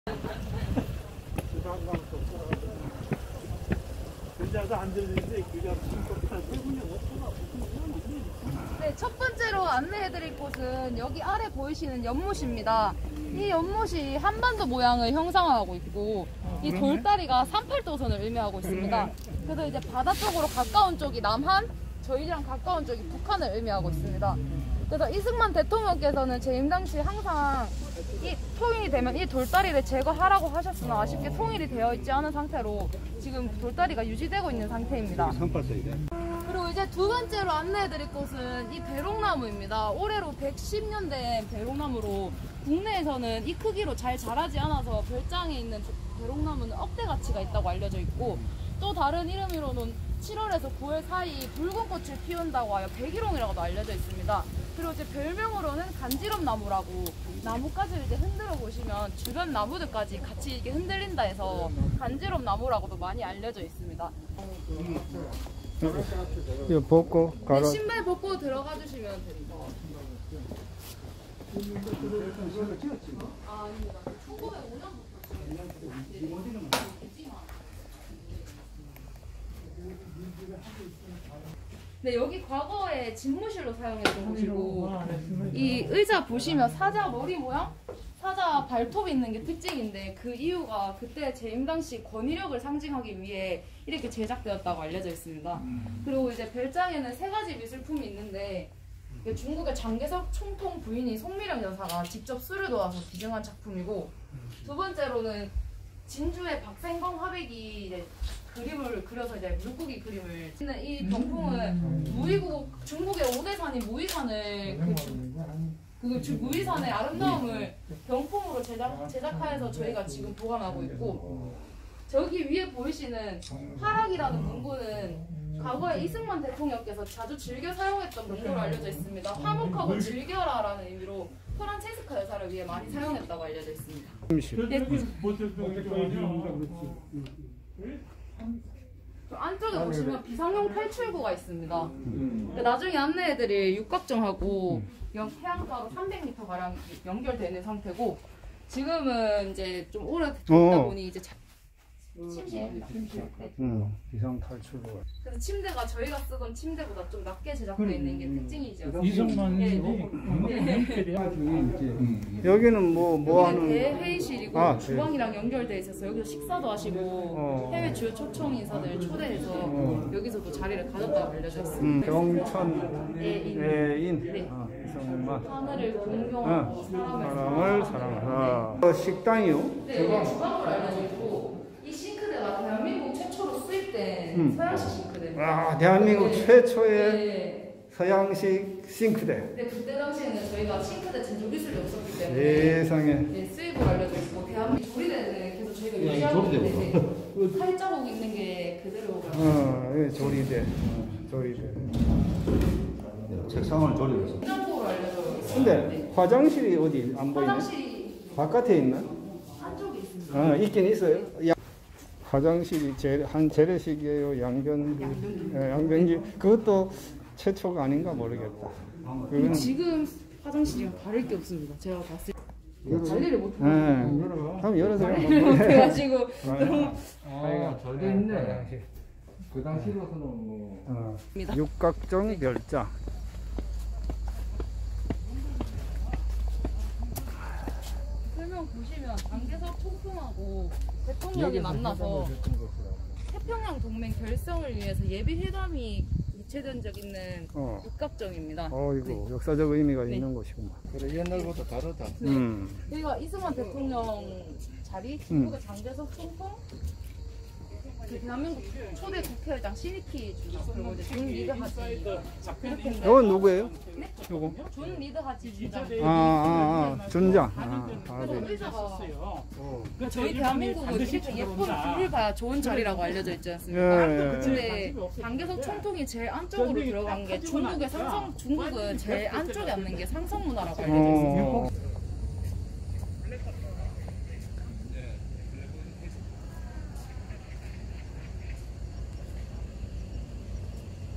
역사적 현장의 장소_진해 해군 기지 내의 이승만 대통령 별장 : 김선규교수
진해 해군기지내에 이승만 대통령의 별장이 있습니다. 상당히 검소하게 지어졌고 집무살, 침실, 주방을 볼수 있습니다. 주방의 씽크대가 한국 최초로 설치된 것이라 합니다. 육각정에서는 태평양 동맹을 위한 예비회담이 열린 곳으로서 장개석 총통외 4인이 회담을 하였습니다.